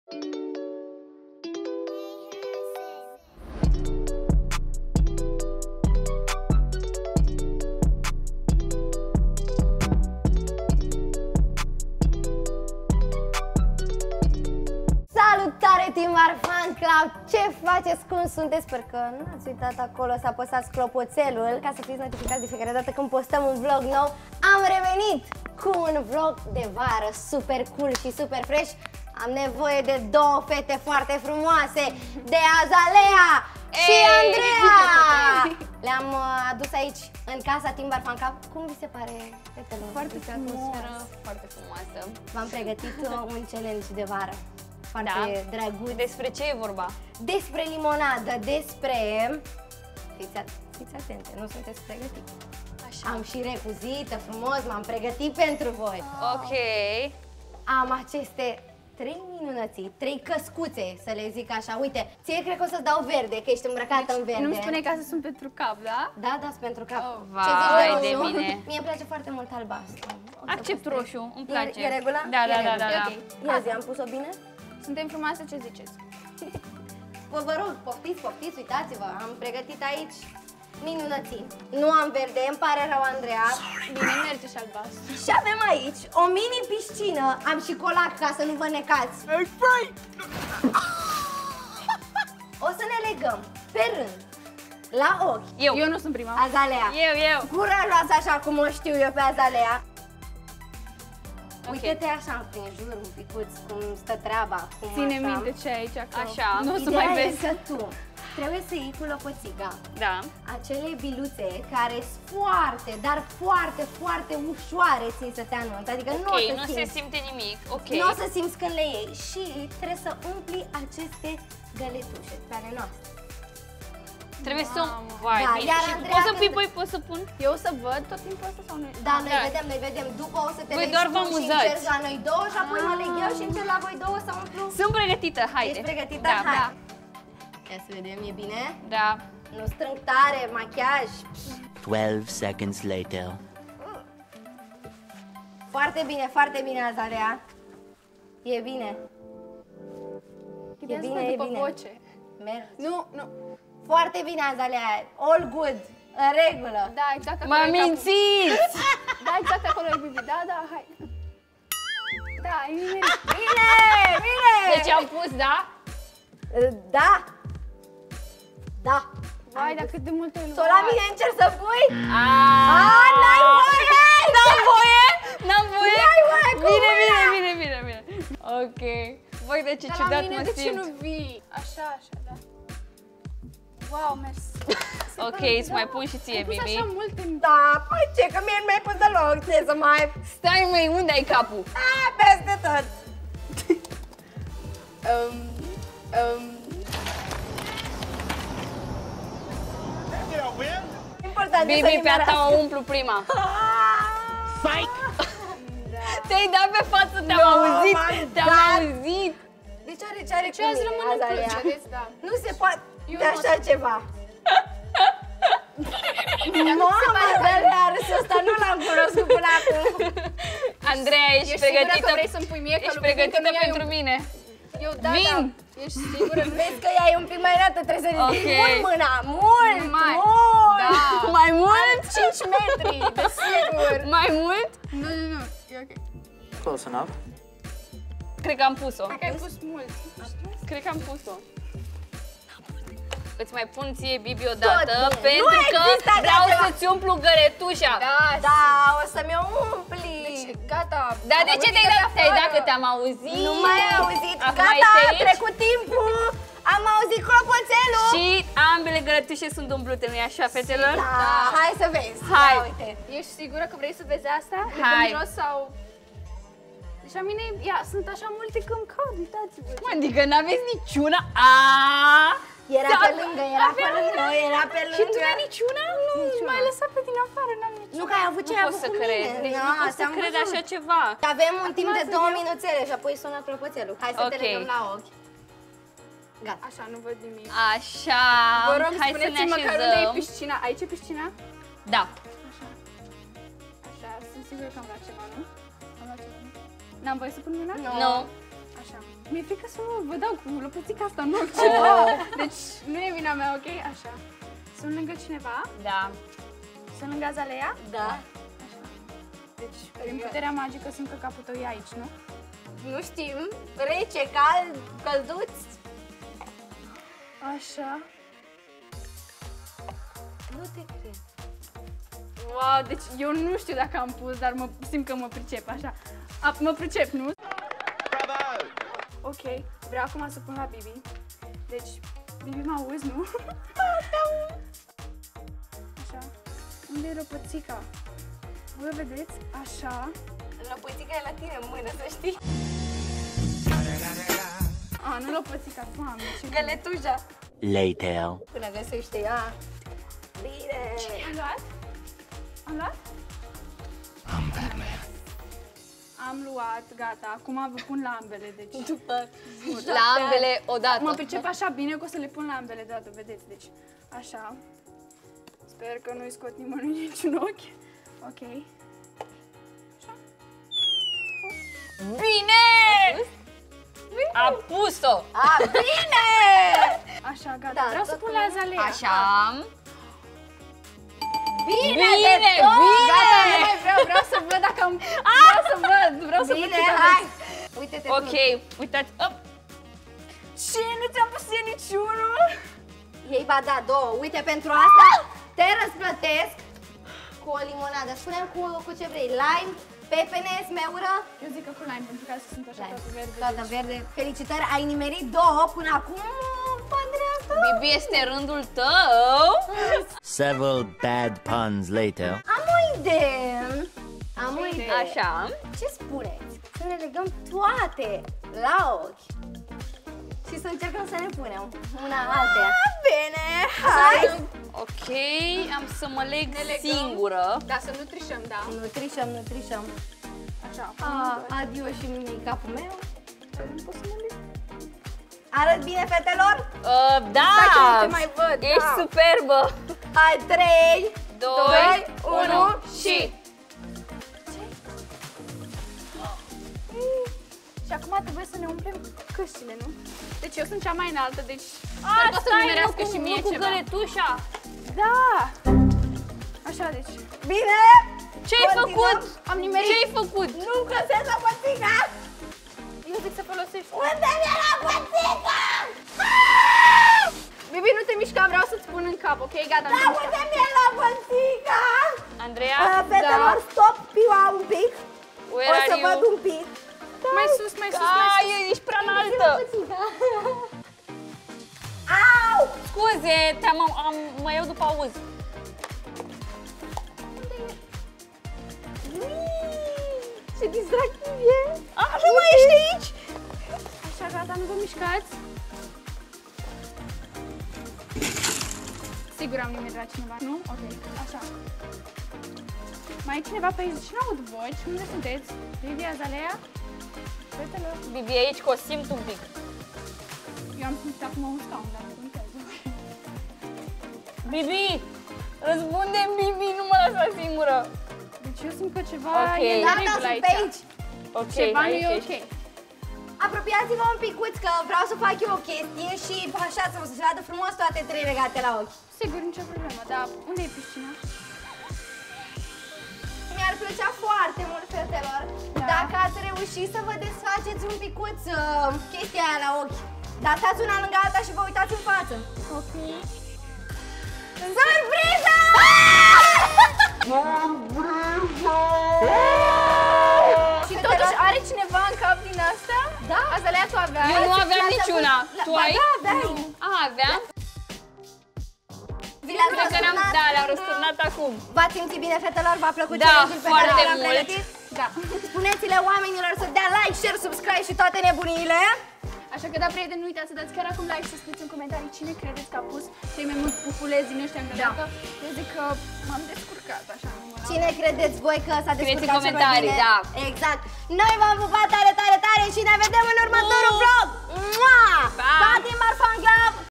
BIBI, AZALEEA ȘI ANDREEA BOSTANICA APROAPE REUȘESC SĂ...
Bibi le arată Andreei Bostanică și Azaleei Necula cum poți să-ți încălzești recuzita de plajă pentru vacanță 🏖️! Vezi cum au reușit să se păcălească între ele, într-un episod ușor, de vară 🏝️🌊.